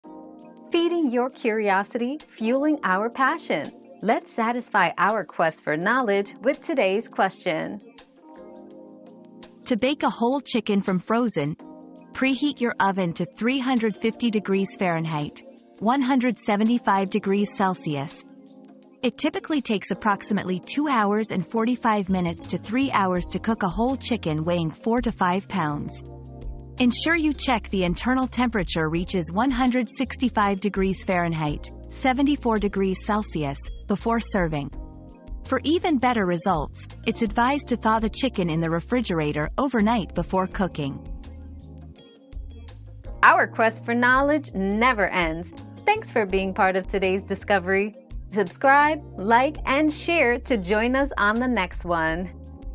How long to bake a whole chicken from frozen?
Bake Whole Chicken from Frozen: Easy Guide 👉 Frozen Chicken Bake 👉 Learn how to bake a whole frozen chicken to perfection in this simple guide. Cook time, temperature, and serving tips included!

Laura S. Harris (2024, May 26.) How long to bake a whole chicken from frozen?
    🌐 AskAbout.video